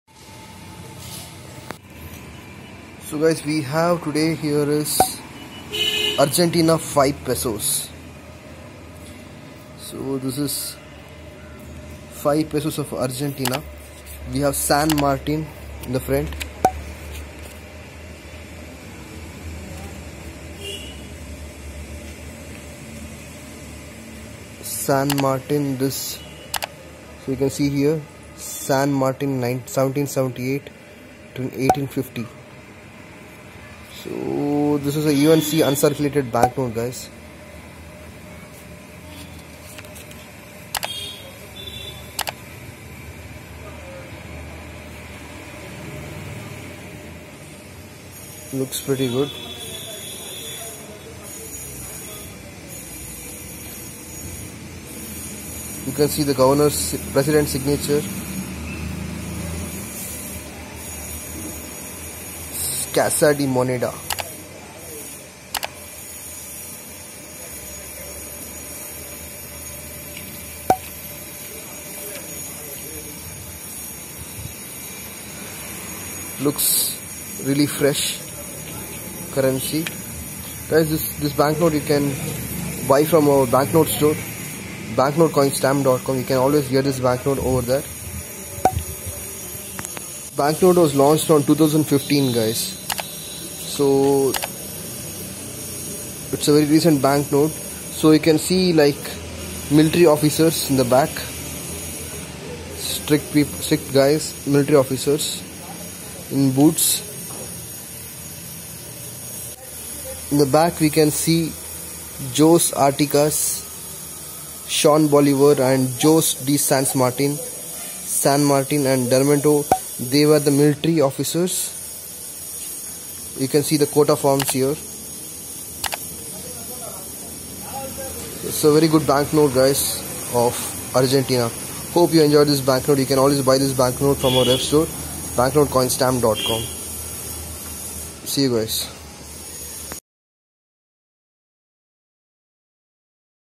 Argentina 5 Peso San Martin 2015
We see Banknote of Argentina 5 Peso in the from we have Military officer San Marin